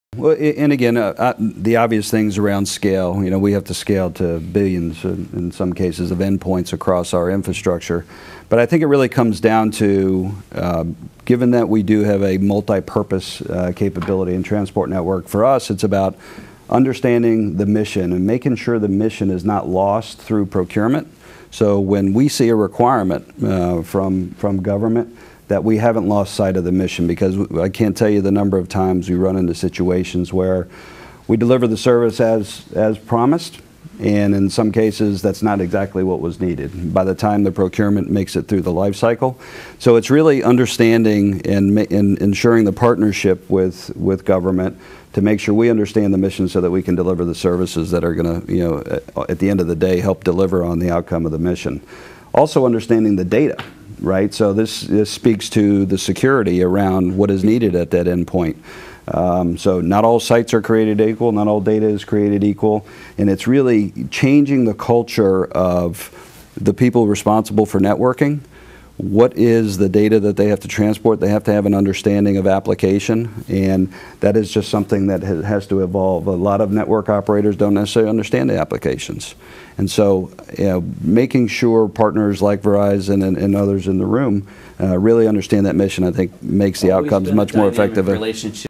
FEF Next Gen Networks Lessons Verizon Oct 2018
Verizon's Steven Lefrancois on Lessons Learned with Next Gen Federal Networks-Federal Executive Forum Program-October 2018